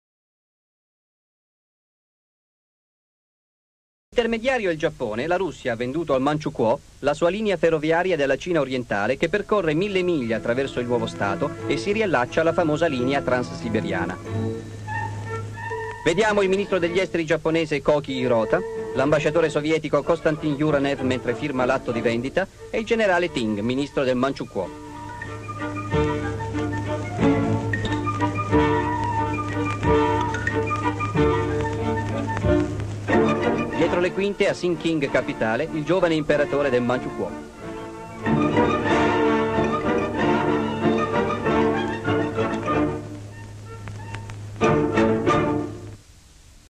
La Russia vende al Manchukuo una linea ferroviaria con l'intermediazione del Giappone
Descrizione sequenze:carta geografica della Cina e del Manchukuo dove è indicato il percorso della linea ferroviaria ; il ministro giapponese Koki Hirota viene fotografato tra la folla ; l'ambasciatore sovietico Yurenev e il ministro dekl Manchukuo Ting mentre firmano l'atto di vendita ; i firmatari si stringono la mano ; il giovane imperatore del Manchukuo seduto alla sua scrivania ;